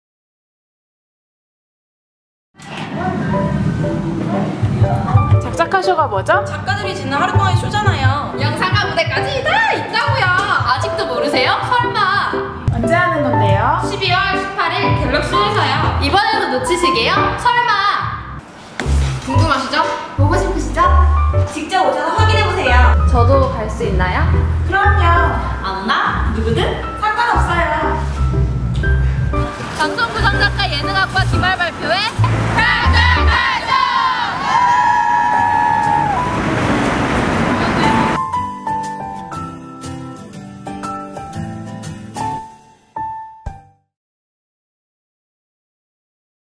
[싹튜브] 영상과 무대까지 SAC 다 있는 방송구성작가 발표회 '작작하쇼'
SBS 스타주니어 붕어빵 메인작가 곽상원교수님, MBC 아빠어디가 메인작가 여현전교수님까지!! 서울종합예술실용학교 최강의 두뇌를 가진 그들이 뭉쳤다!!

작작들이 짓는 하루동안의 SHOW
12월 18일 갤럭시홀~!! 이번에는 놓치지 마세요~~~